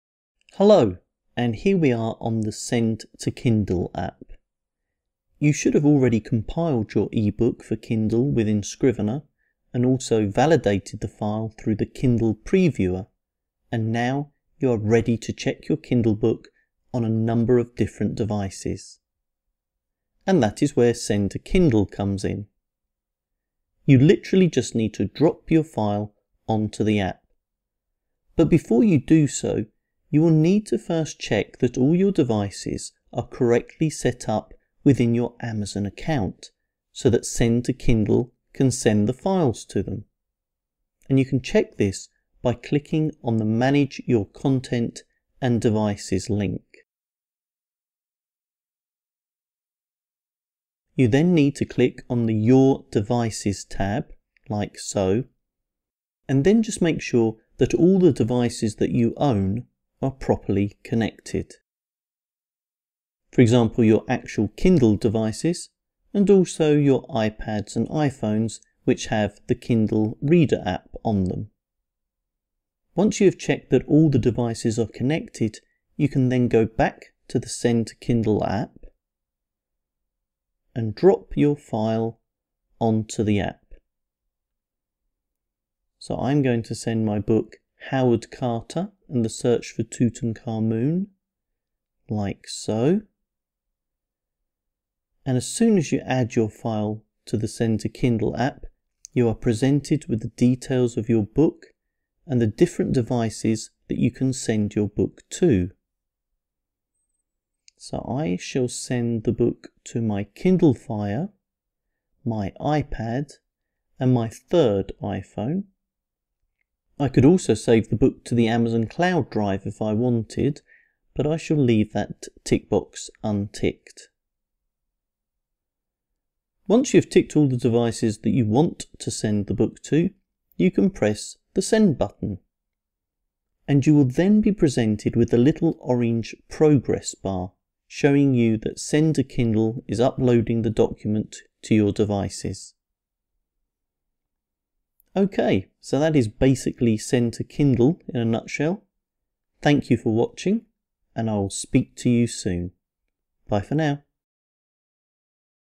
How to use the Send To Kindle application
This video shows you how to use Amazon's Send To Kindle application to send an ebook to any of your Kindle devices for testing that the formatting of your book is correct.